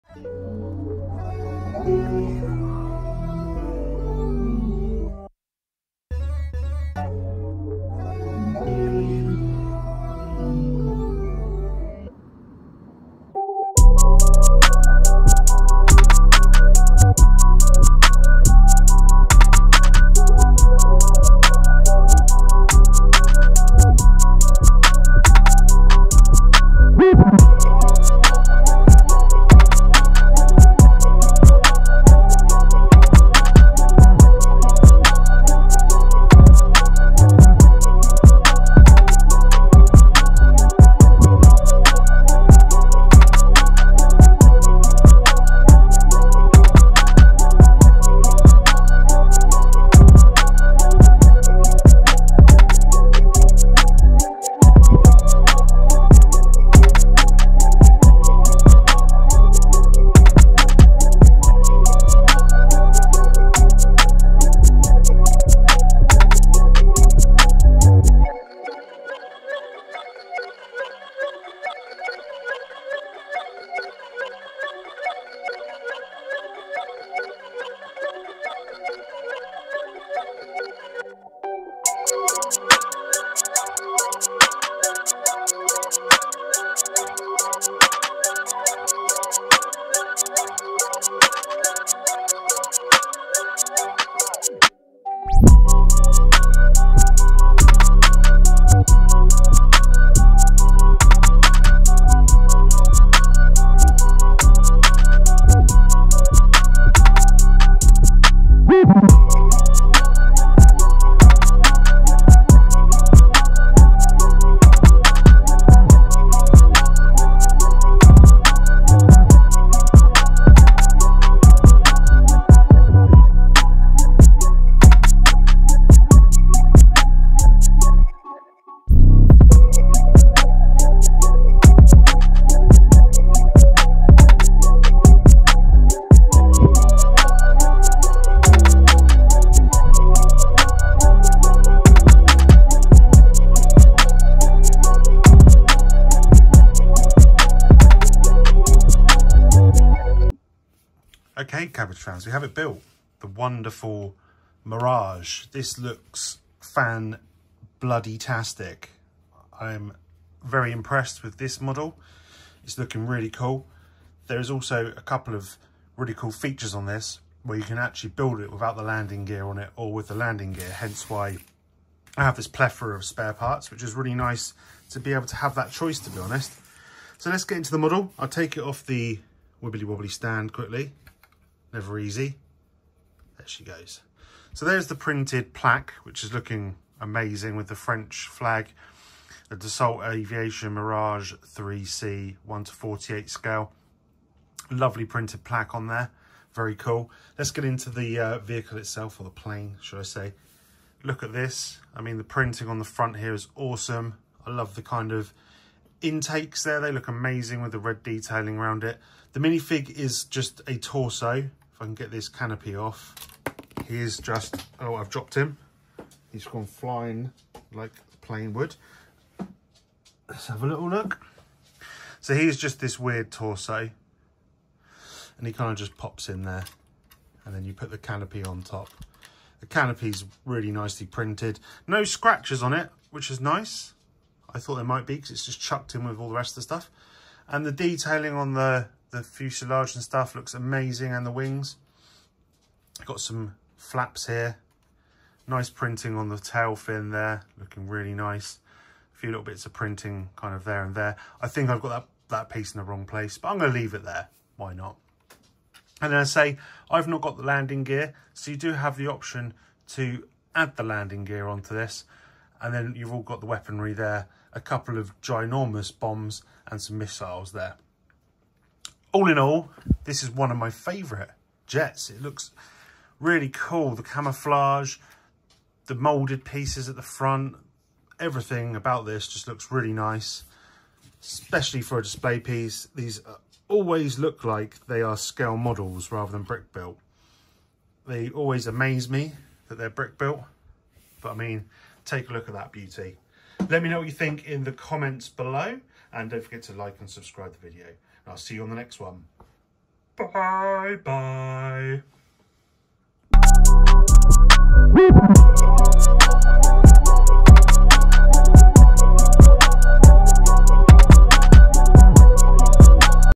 Mirage IIIC - Cobi 5818 - Review
Mirage IIIC - Cobi 5818
444 Pieces
Half a Minidude
1:48 Scale
Low Skill Build
9 out of 10 Sprouts